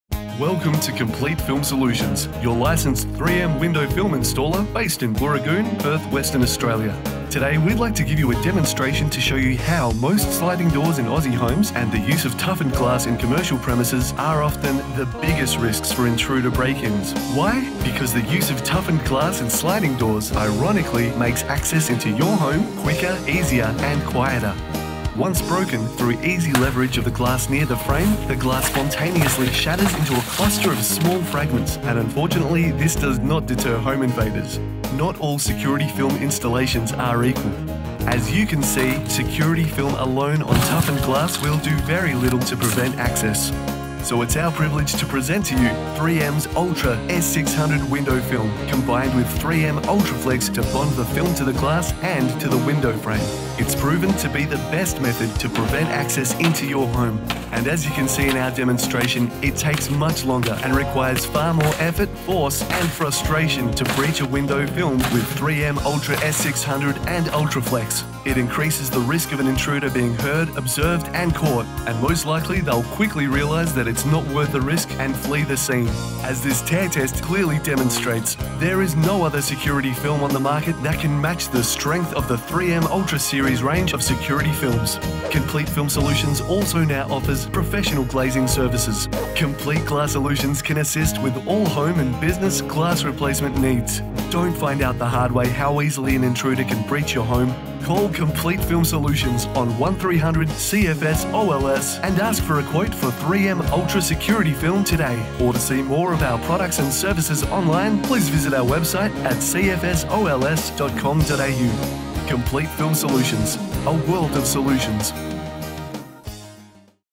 3M Ultra S600 Security Film Demonstration (Complete Film Solutions- 2019 update)
Complete Film Solutions demonstrates the effectiveness of 3M Ultra S600 clear security film installed to toughened glass with Ultraflex bonding attachment system, and shows why security film alone is not enough for any panels of toughened glass.

(2019 update edit to show new frontage of CFS premise in Booragoon, Western Australia).